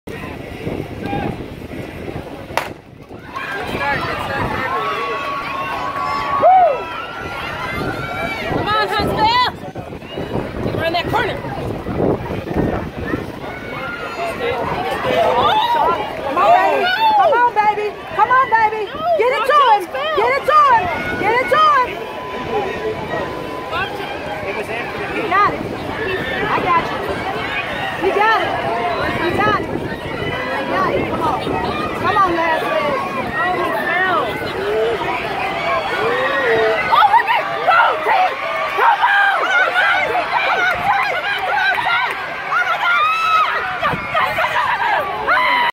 AHSAA Sectional Finals @ James Clemens 7A Boys 4×100m
Sparkman Lane 3 First Place Finisher Chase Parker,  Charles Lewis, Hunter Gibson, and Carlos Burwell